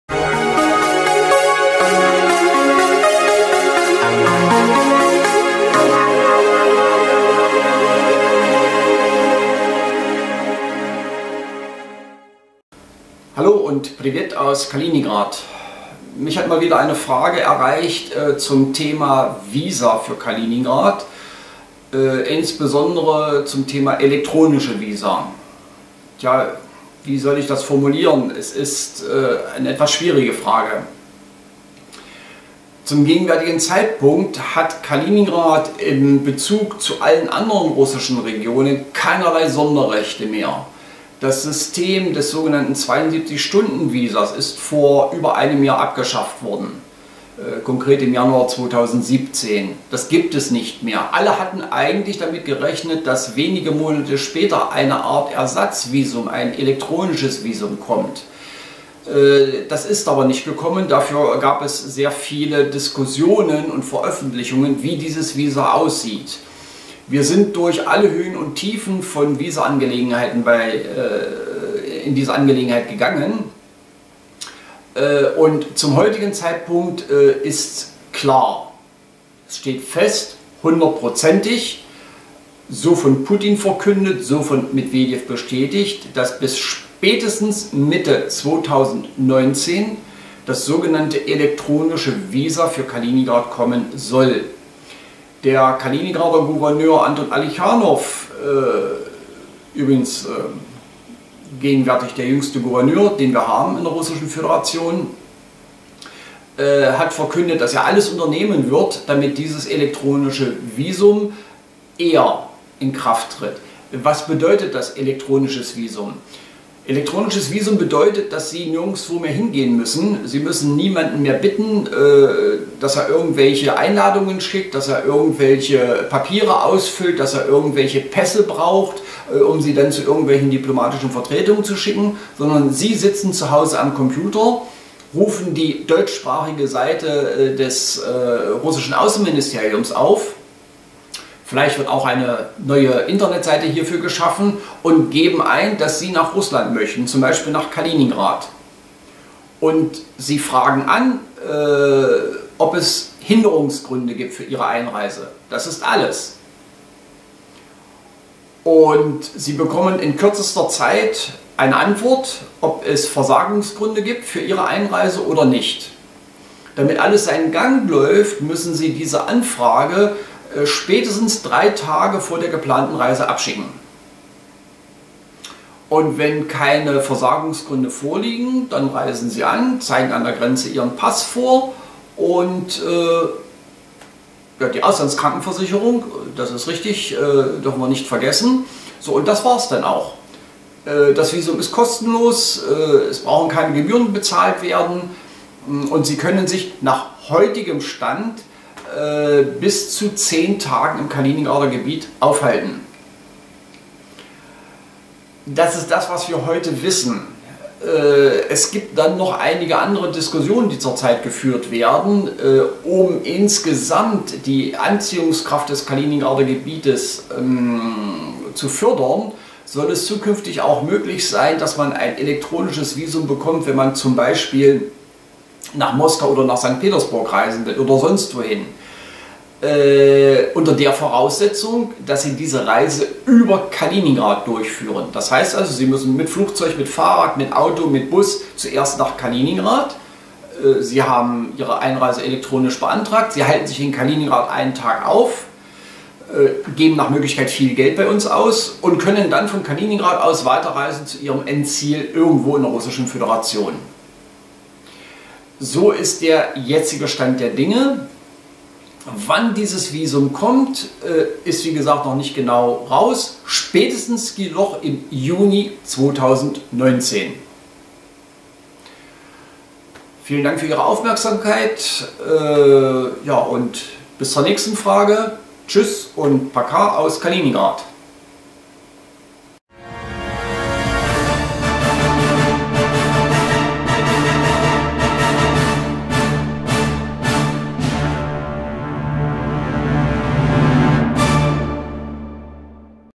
Kaliningrad: Elektronisches Visum
Gegenwärtig gibt es zu Visafragen keinerlei Sonderregelungen für das Kaliningrader Gebiet. Das ehemals beliebte 72-Stunden-Visum ist seit Januar 2017 eingestellt worden. Das neue geplante elektronische Visum soll nach bisheriger Planung und offizieller Ankündigung Mitte 2019 kommen. Was man unter elektronischem Visum versteht und welche Informationen rund um dieses Visum gegenwärtig existieren, wird in diesem Beitrag aufgezeigt.